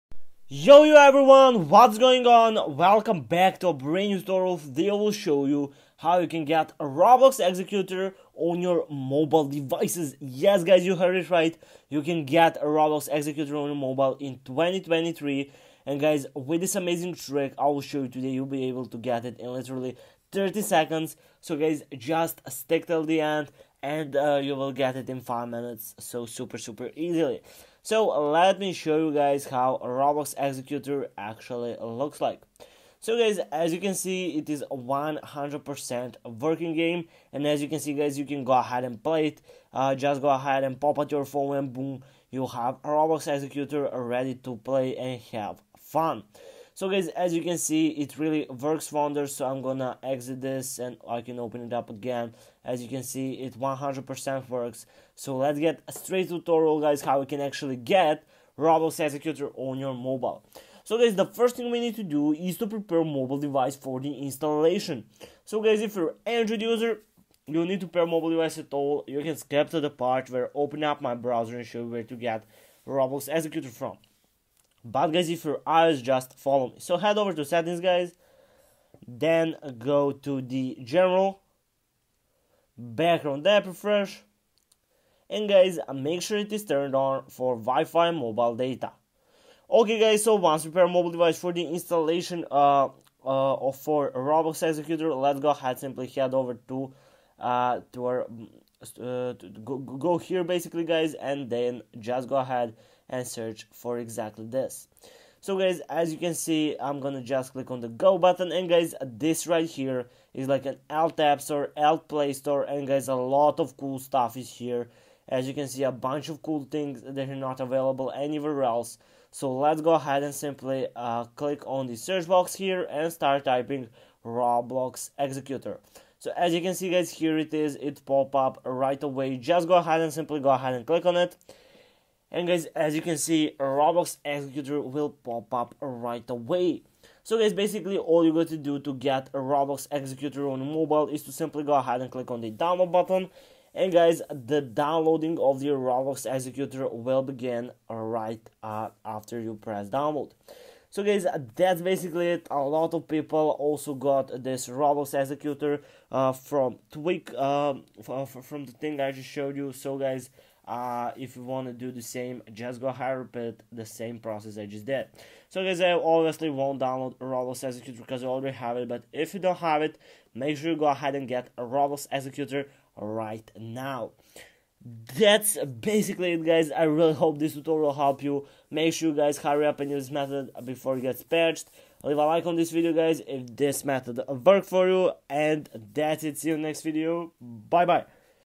Roblox Executor Mobile Download - How to Download Roblox Executor on iOS & Android
In this video, we'll show you how to download Roblox Executor on iOS & Android devices. Roblox Executor is a powerful tool that allows you to manage and publish your Roblox games on the Roblox website.

If you're looking to download Roblox Executor on your iOS or Android device, then this video is for you! We'll walk you through the process of downloading and installing Roblox Executor on your device. After watching this video, you'll be able to effortlessly manage and publish your games on the Roblox website!